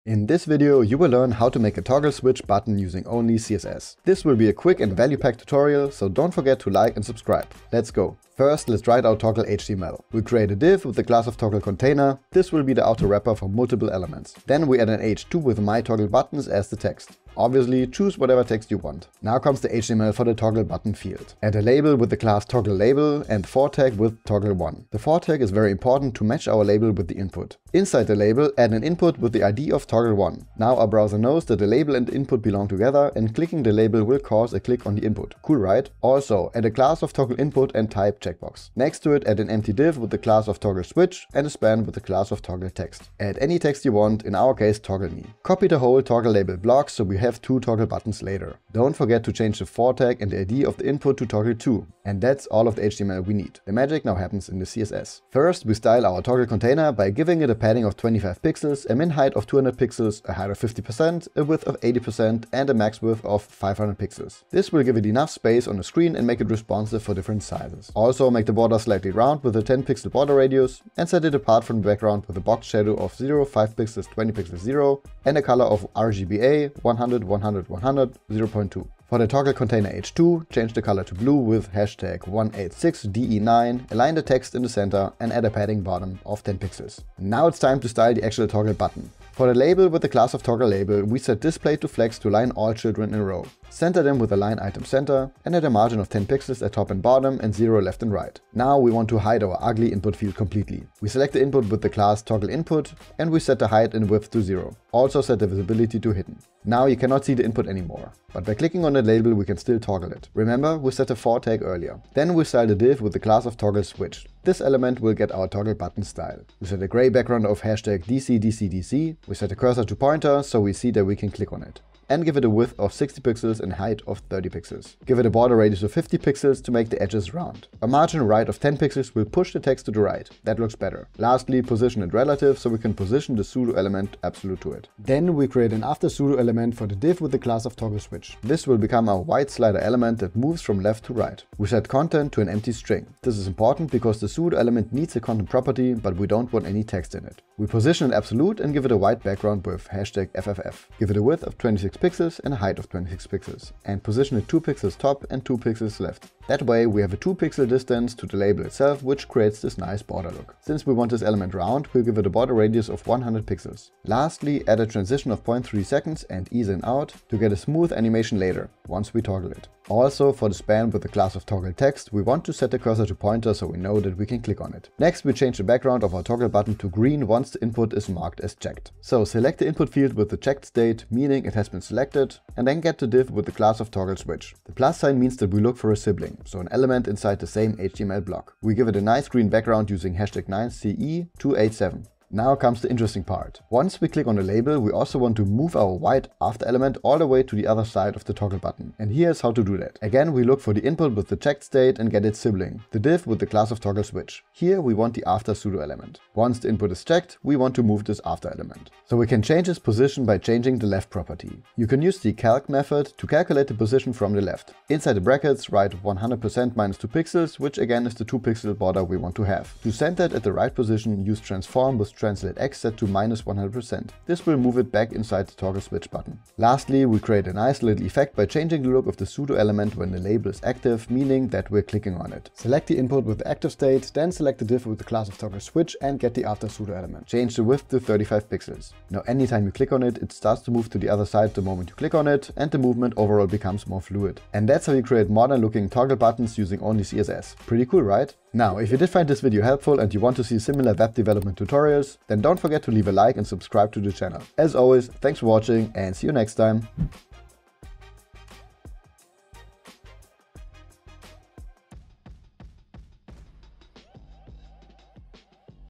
Make Toggle Buttons - Only CSS
In today’s video, you will learn how to make modern-looking toggle buttons using only CSS.

Check out the Codepen below for the full code.

Happy Coding!

Timestamps

00:00 | Intro
00:09 | HTML - Container
00:19 | HTML - Toggle Button
00:55 | CSS - Container
01:29 | CSS - Toggle Button
03:20 | CSS - Toggle Animation
04:04 | Outro